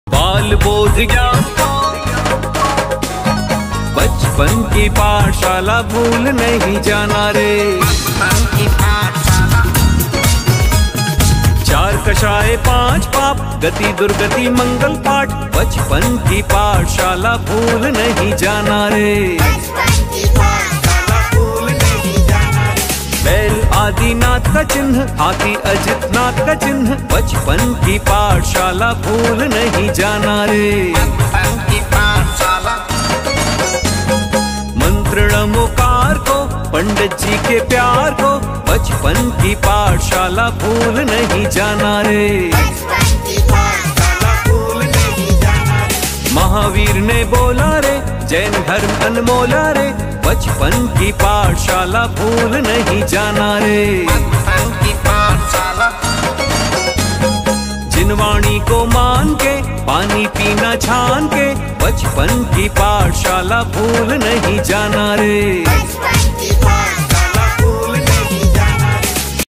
Bachpan ki Pathshala
Kshama Vanee 2021
Presented by
Shree Adinath DIgamber jain Pathshala Thane
Programme organise by
Shree vidhya vanee music group thane